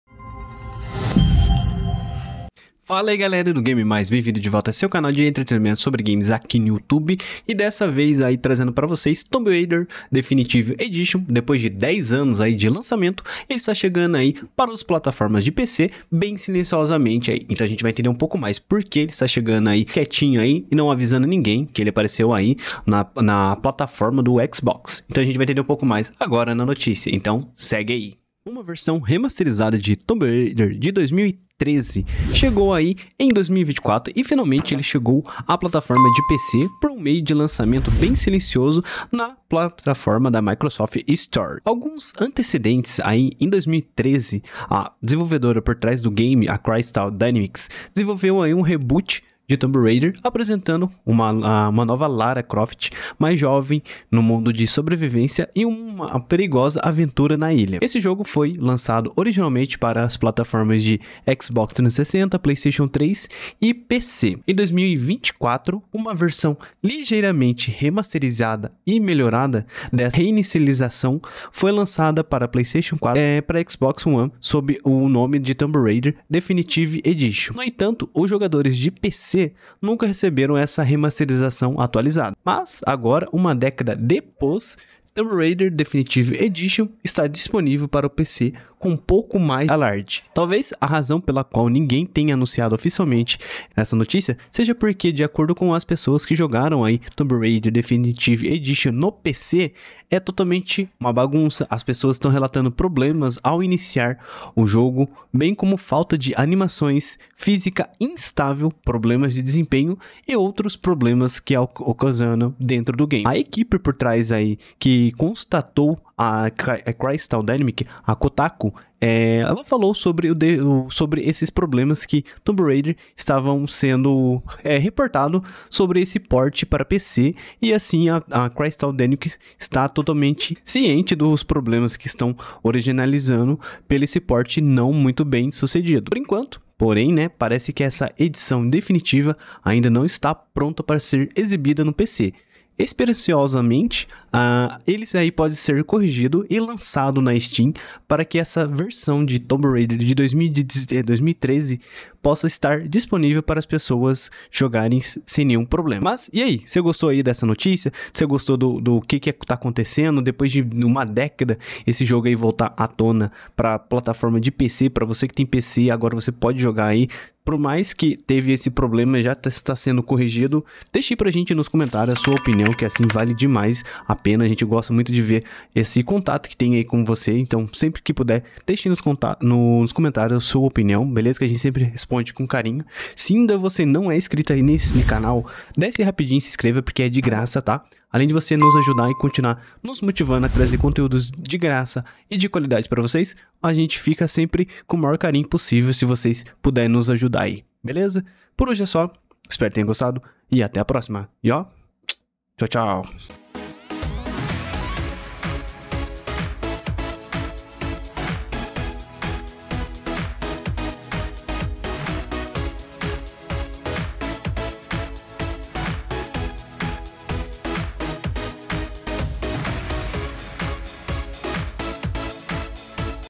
Após uma década, Tomb Raider: Definitive Edition finalmente chega ao PC
No entanto, a parte que pode ser considerada negativa por certos jogadores é que Tomb Raider: Definitive Edition no PC está disponível somente por meio da Microsoft Store. Essa versão do jogo não existe no Steam.

Planos Disponíveis
Prata: 4.90 por mês
Ouro: 19.90 por mês
Platina: 99.90 por mês